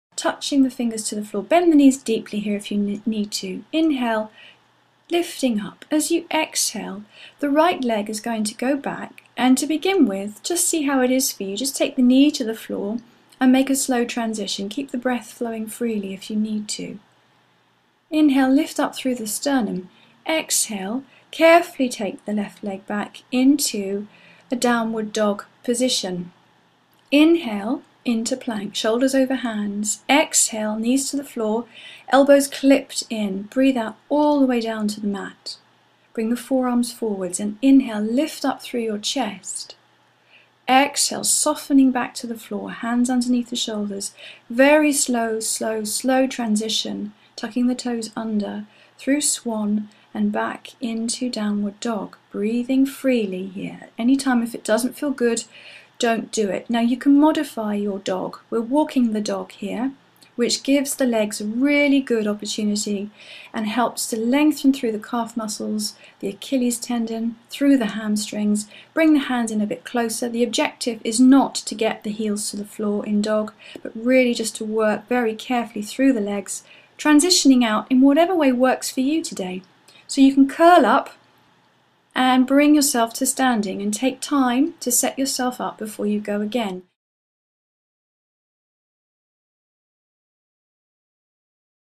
Yoga Sequence Bringing it All Together.mov
Final Training Sequence:  this final week's training session  brings together everything that you have experienced with us. This week's training covers all the areas you need to stay fit and keep injury free!
To learn more go to Bronze Page.